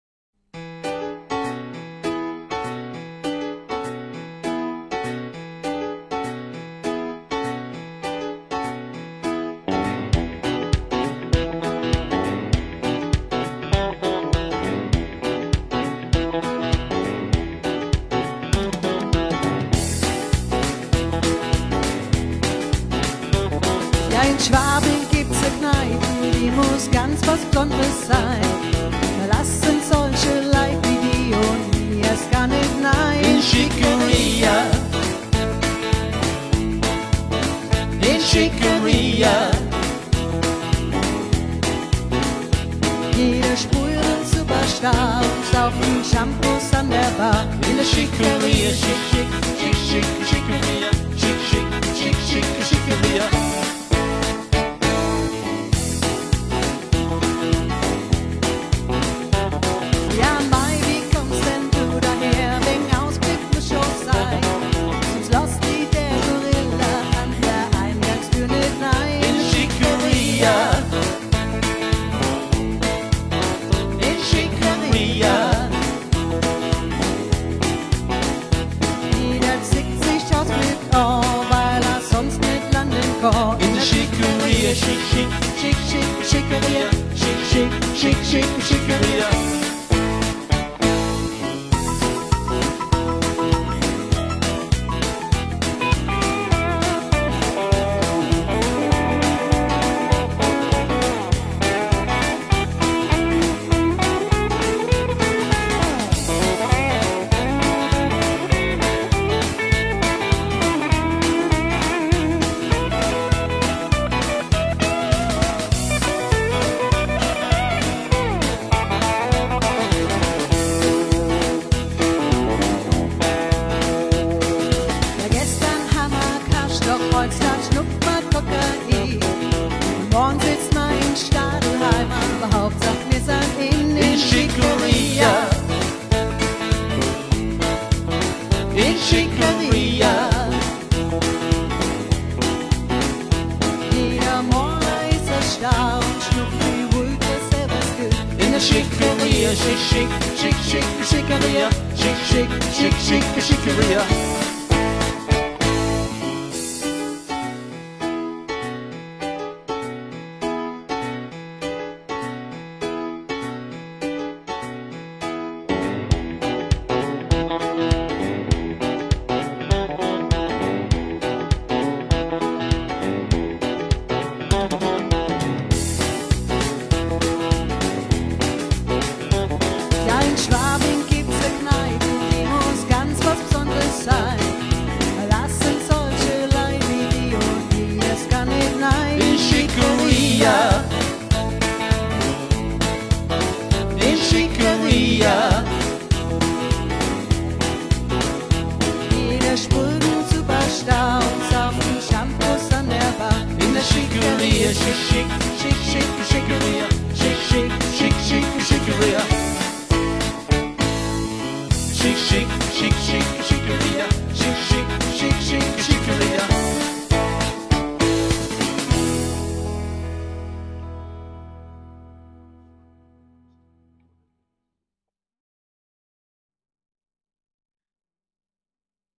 Schickeria - Facebook Charity Live Stream for Saarlouis Dementia Day Care Center
A clip from our live stream on facebook (29/05/20), trying to raise funds for The Dementia Day Care Center in Saarlouis here in Germany. (Sorry about the video quality, it wasn't recorded as it was streamed, so the video was grabbed low quality from facebook)